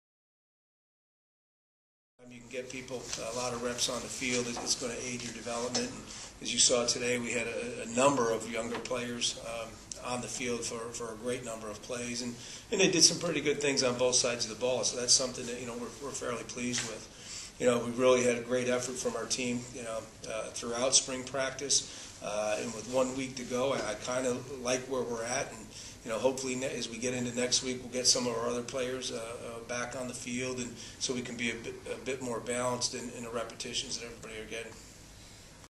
Coach Callahan Interview post spring scrimmage on 4/22/11
Monmouth Head Coach Kevin Callahan gives an opening statement after first scrimmage of the spring 2011 season (4/22/11)